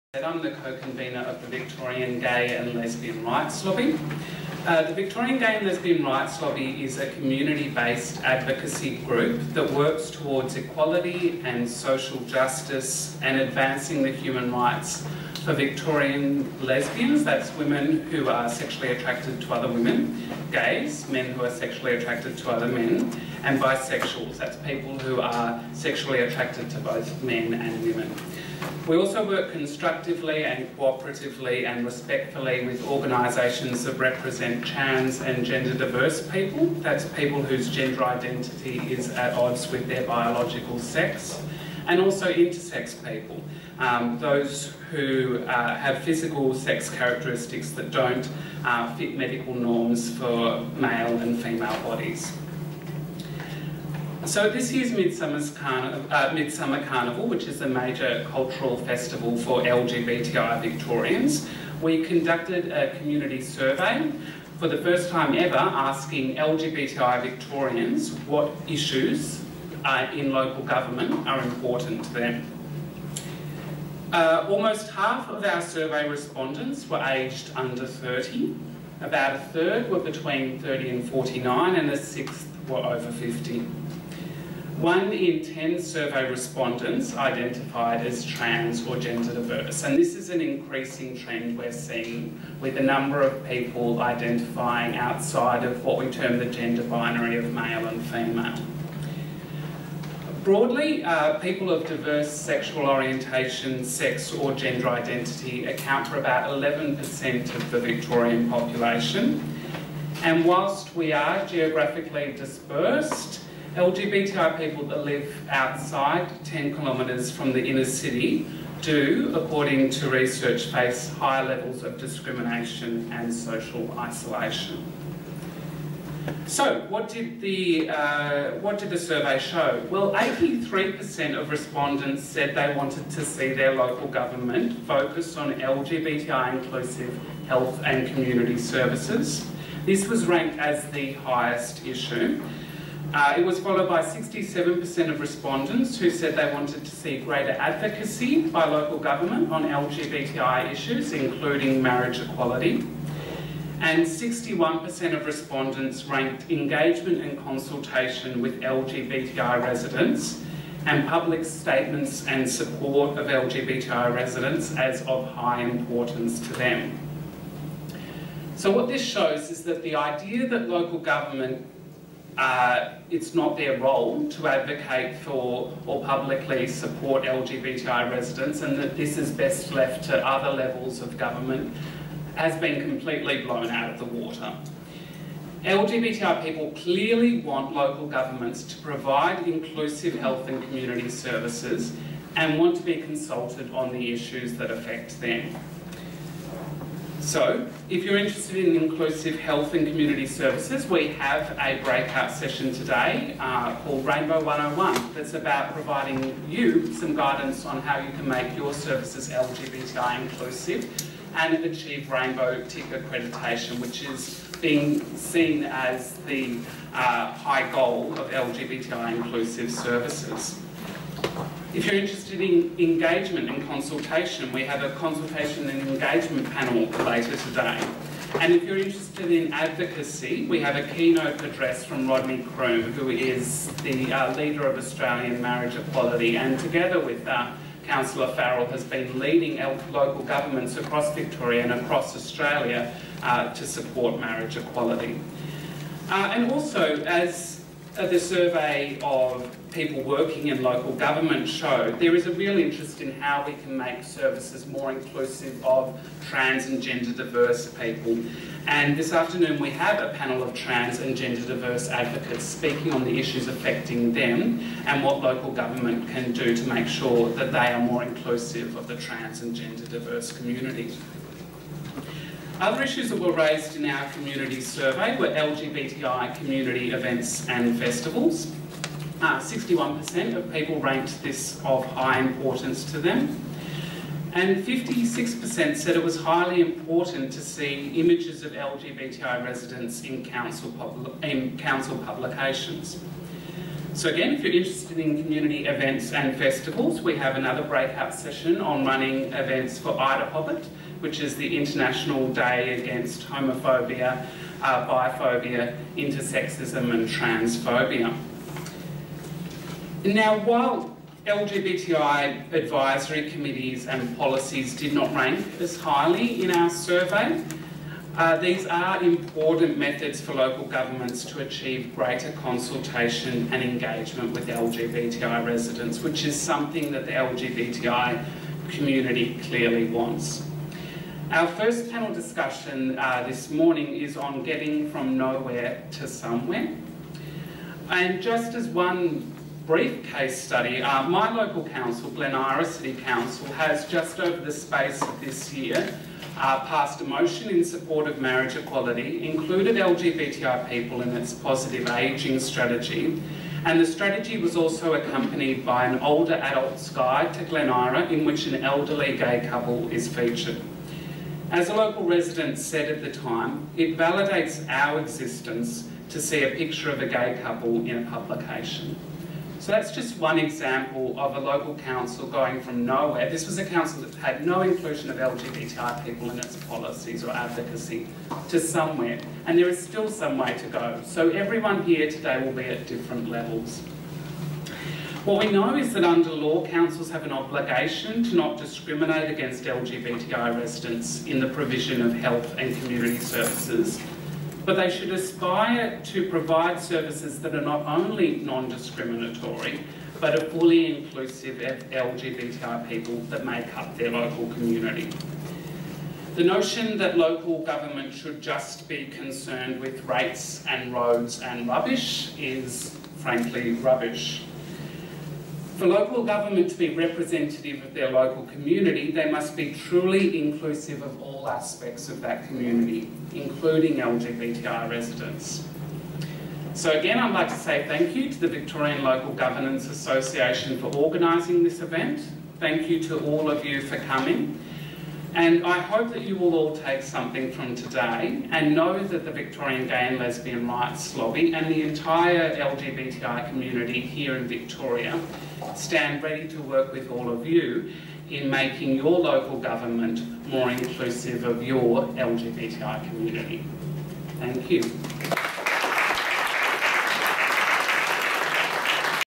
Sean Mulcahy - Local Government Turns Rainbow
Sean Mulcahy of the Victorian Gay and Lesbian Rights Lobby speaking at the VLGA's 2016 Local Government Turns Rainbow Forum